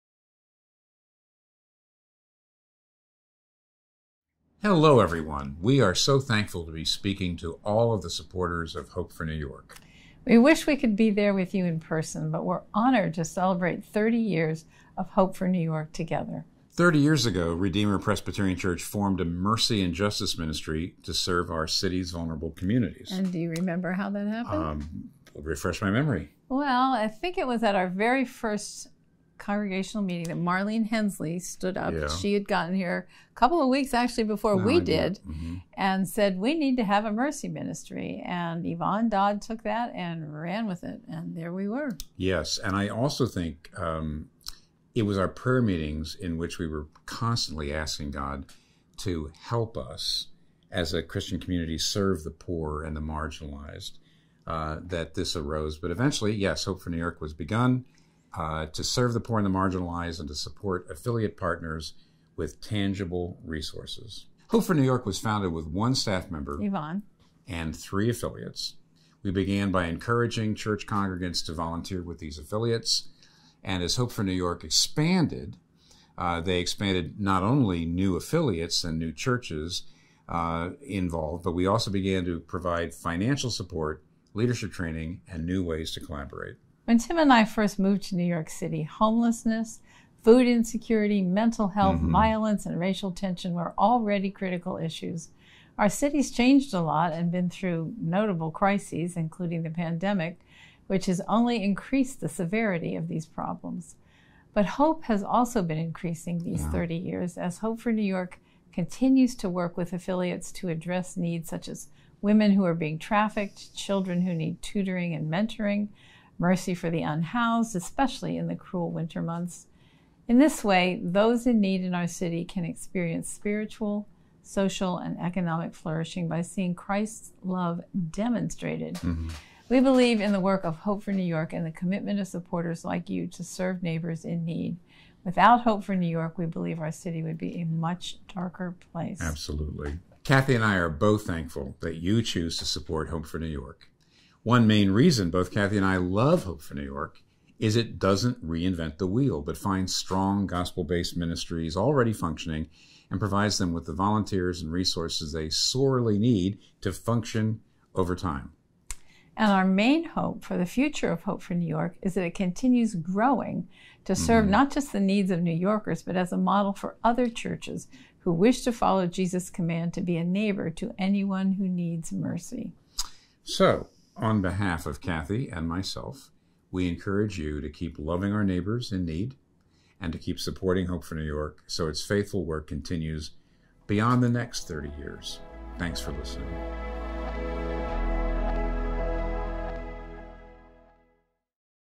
A Special Message from Rev. Tim and Kathy Keller
At our Fall Benefit 2022, both Rev. Tim Keller and Kathy Keller shared a special message celebrating our 30th anniversary!